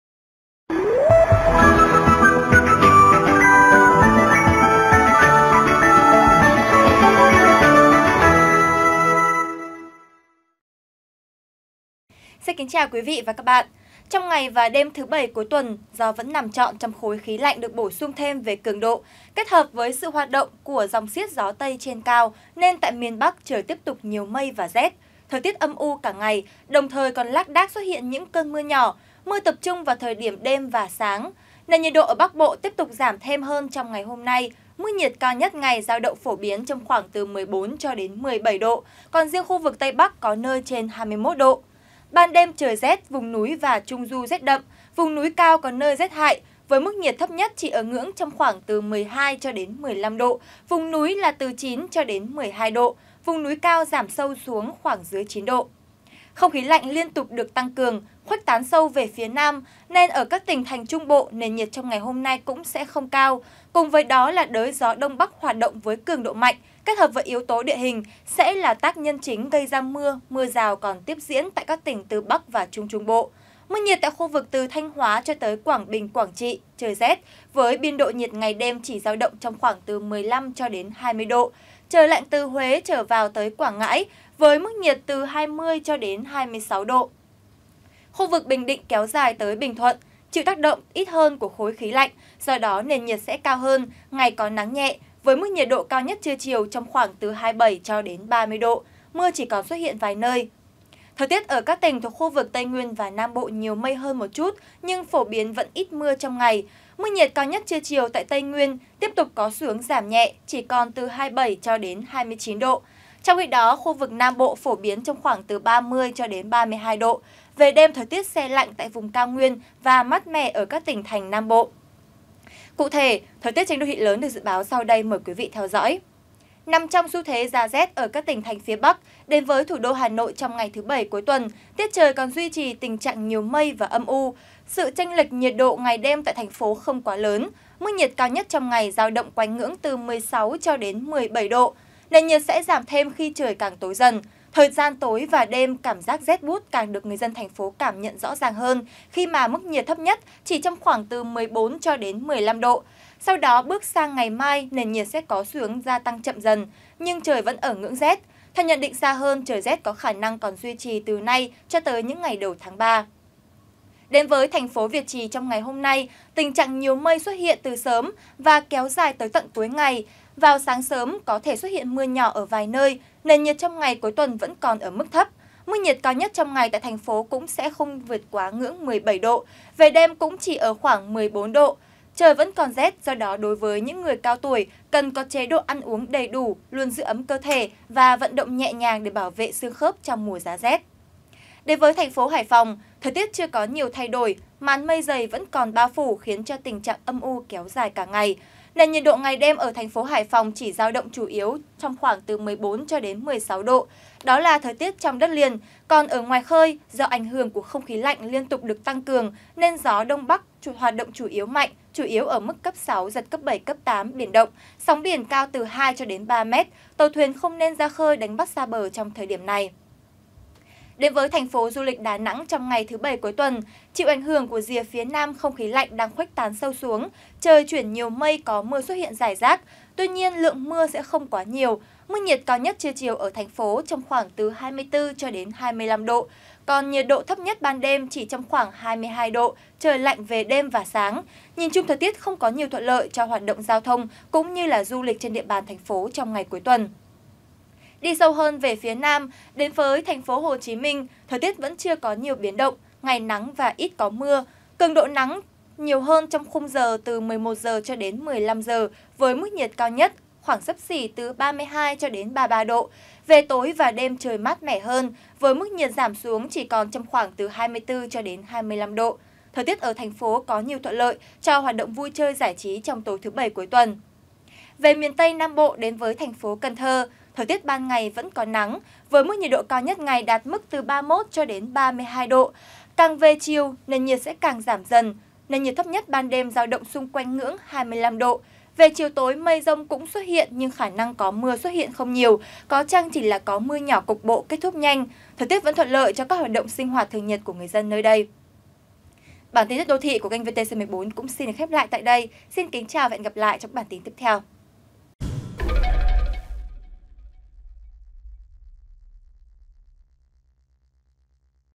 (VTC14)_Thời tiết các thành phố lớn ngày 25.02.2017
(VTC14) - Kính mời quý khán giả theo dõi dự báo chi tiết các thành phố: Hà Nội, Hải Phòng, Đà Nẵng, TPHCM, Cần Thơ ngày 25/02.